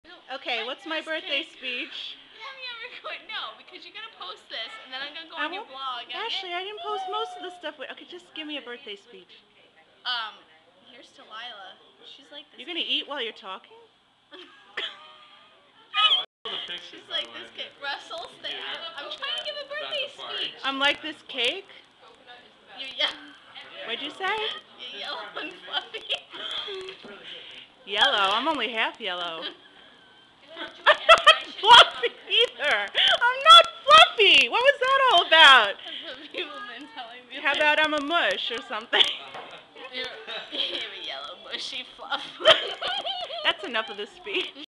Birthday Speech to a Half Asian
My friend recently gave me this birthday speech.  I am half Chinese.